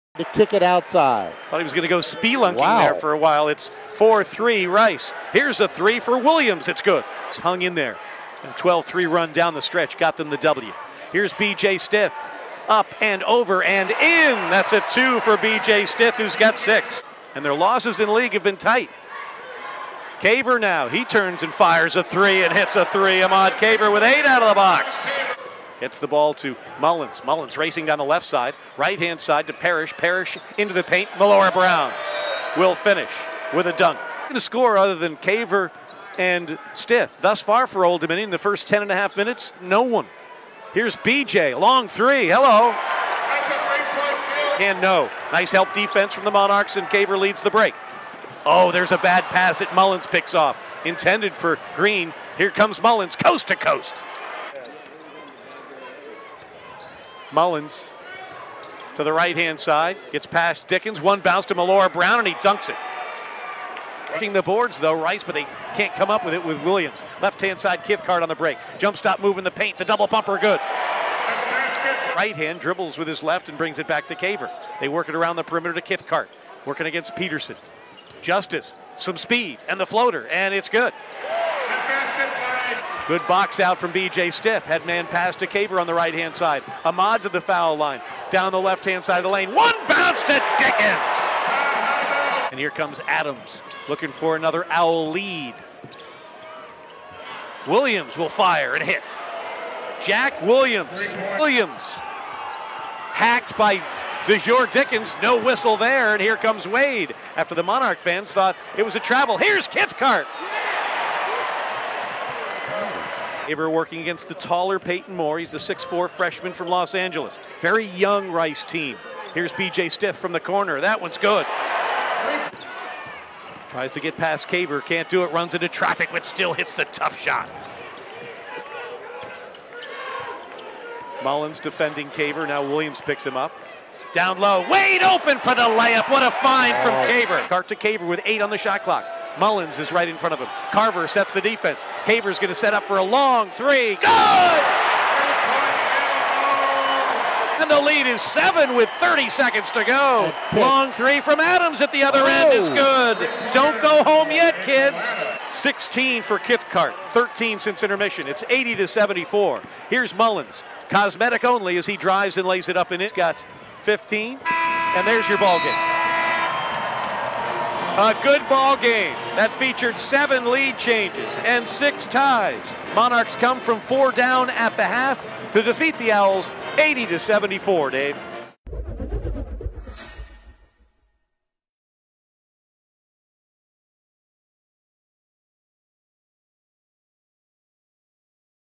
Rice vs. Old Dominion Basketball Highlights (2018-19) | Stadium
Rice vs. Old Dominion basketball highlights from 2019 game. Old Dominion took down Rice 80-76. B.J. Stith notched a season-high five 3-pointers. Stith finished with 28 points and seven rebounds.